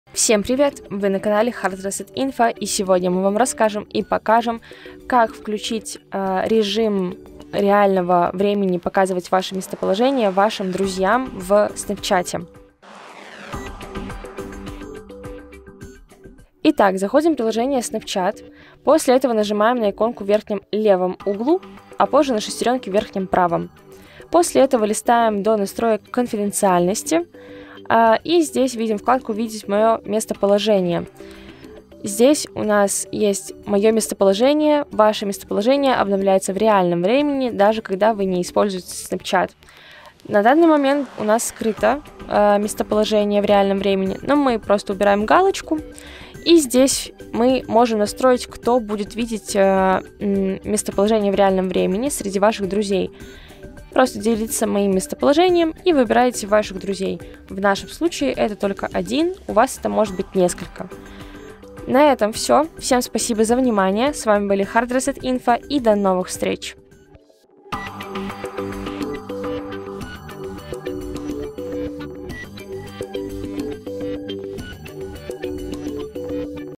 Режим реального времени для друзей в Snapchat | Трансляция геолокации в Snapchat
Добро пожаловать на канал! Не знаете как включить функцию трансляции геолокации для друзей в Snapchat? В этом ролике мы показали как это сделать быстро и легко! Скорее смотрите видео.

Как включить геолокацию в Snapchat? Как влючить функцию  реального времени для друзей в Snapchat? Где включается геолокация в Snapchat?